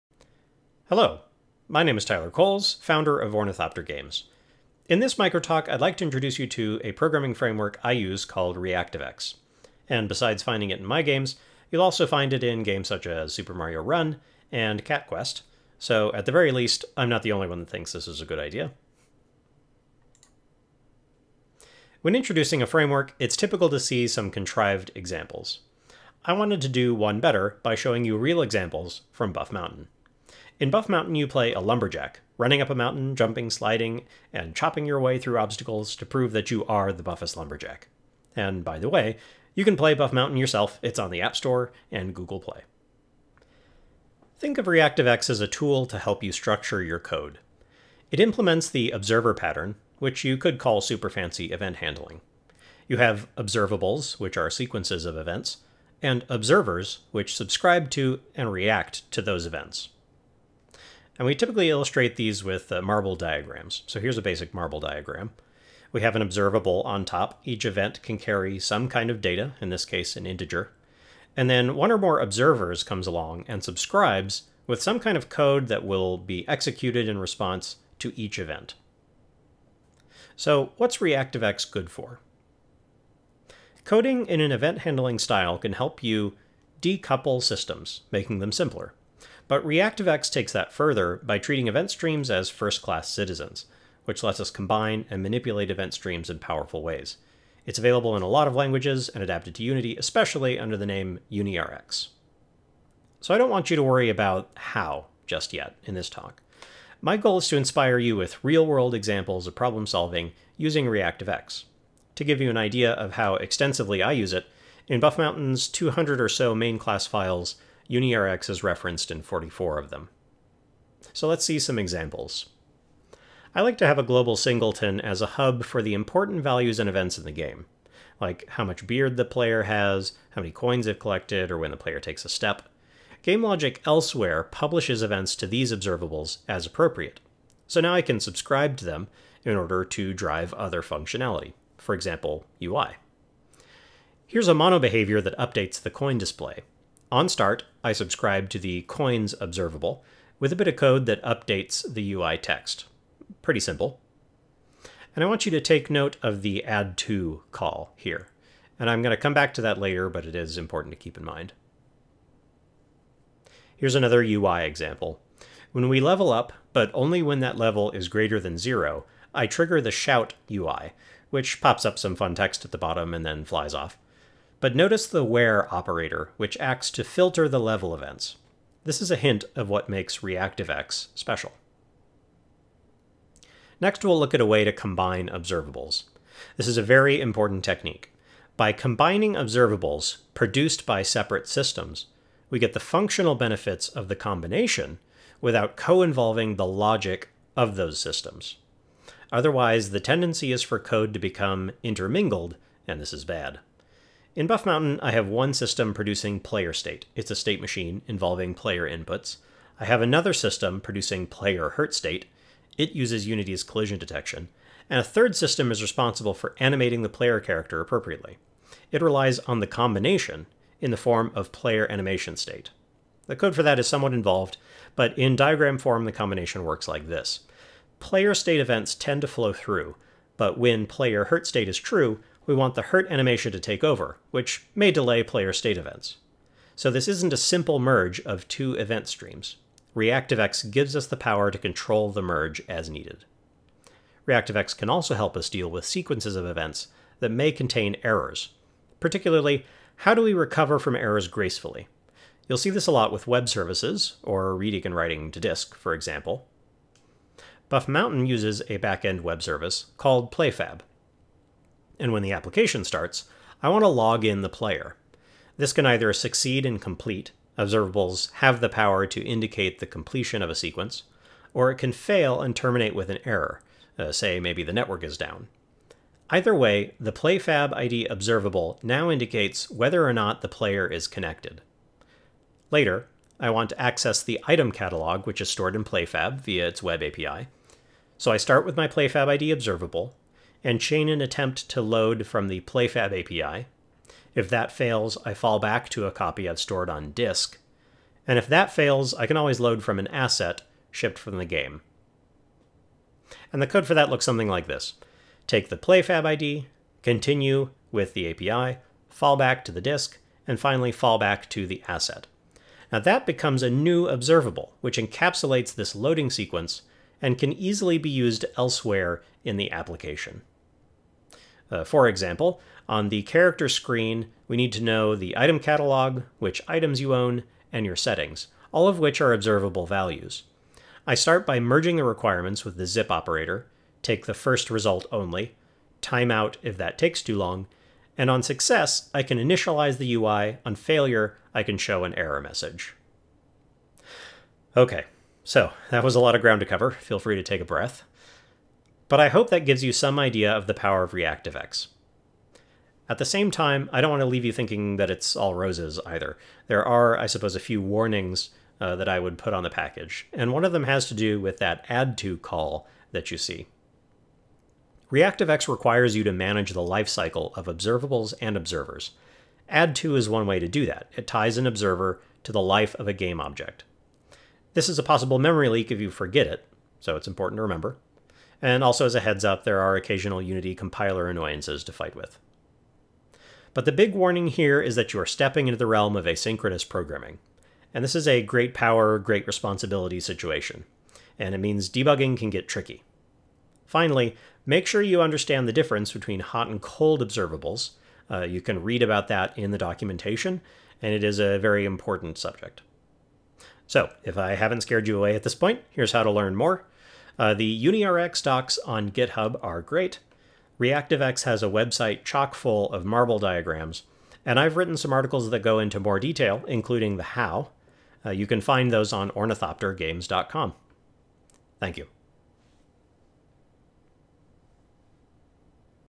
ReactiveX for a reactive axe: real use cases from Buff Mountain
A micro-talk introduction to ReactiveX using real examples from Buff Mountain: the lumberjack endless runner and beard simulator! After this talk, you should have some idea of what ReactiveX can do.

Code examples focus on the ReactiveX implementation for Unity: UniRx.

=== Links ===